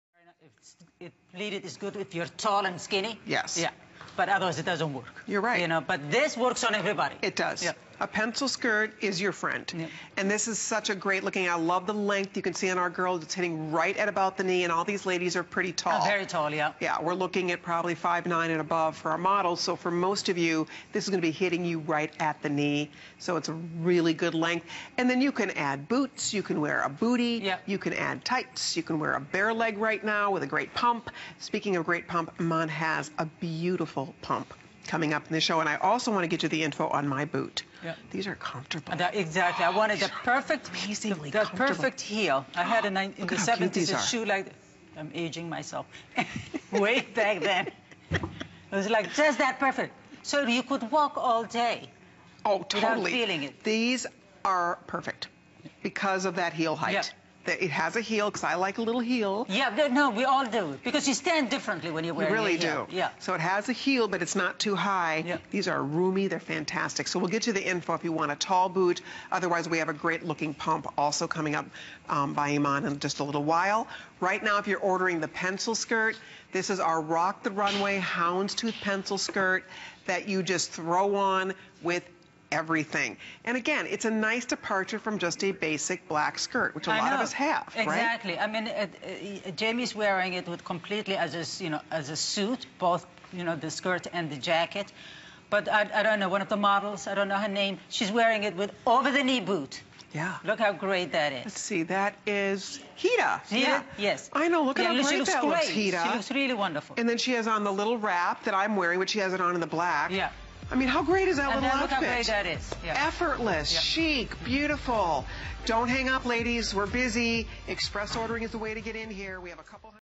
IMAN Global Chic Rock the Runway Houndstooth Pencil Skirt
Get the best of both worlds when wearing this sexy pencil skirt. The mixture of ponte and woven knits provide a comfortable feel and...